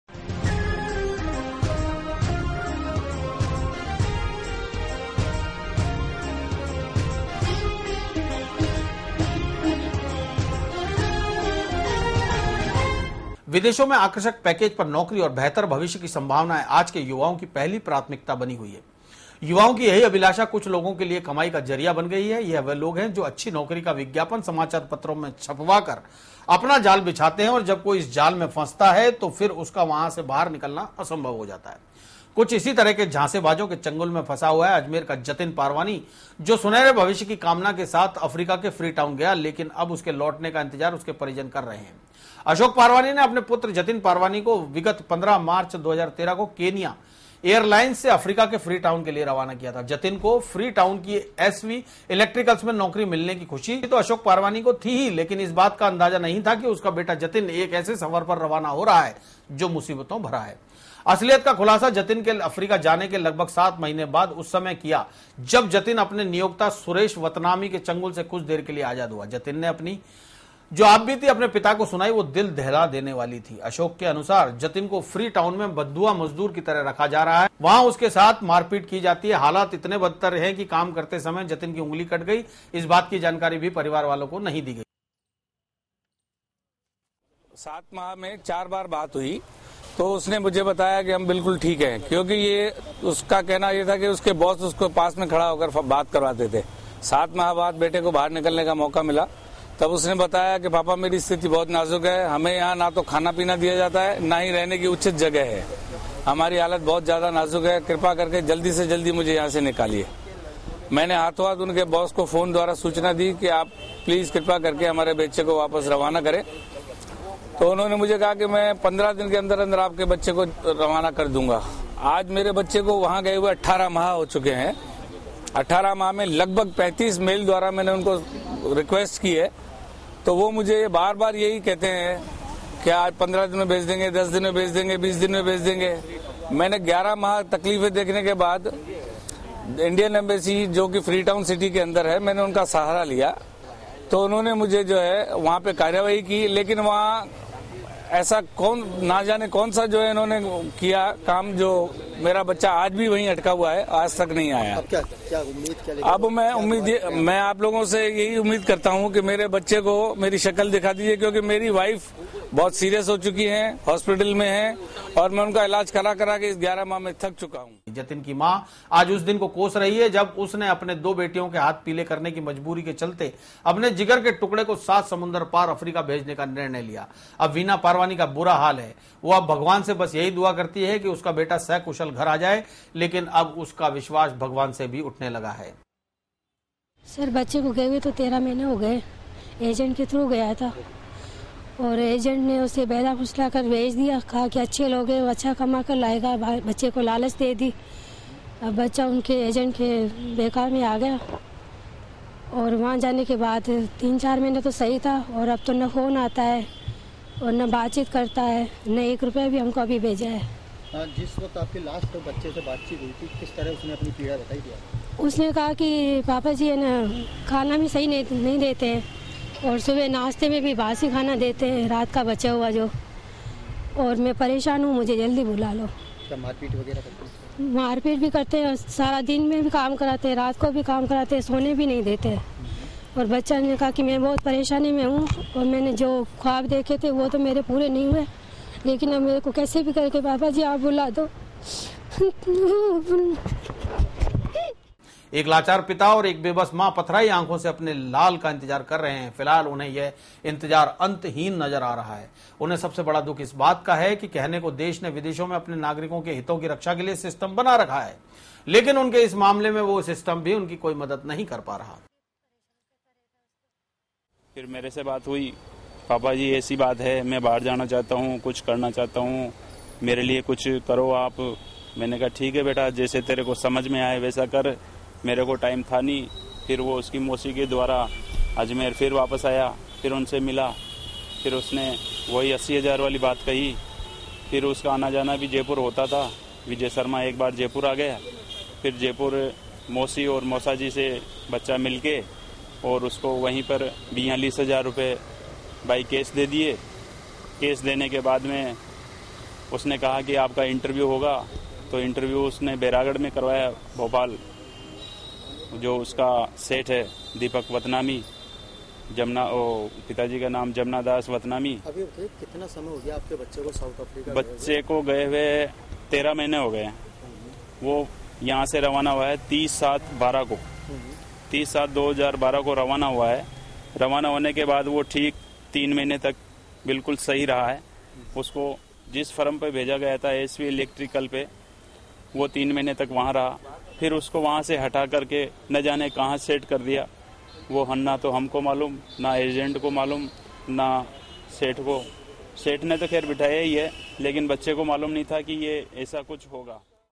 अखबार के विज्ञापन ने छीनी एक परिवार की खुशियां
अखबार के विज्ञापनो की लालच ने हसंते खेलते परिवार को दिये आंसु। साउथ अफ्रीका के फ्री टाउन मे गये युवकों का महिनो से नही है अता पता।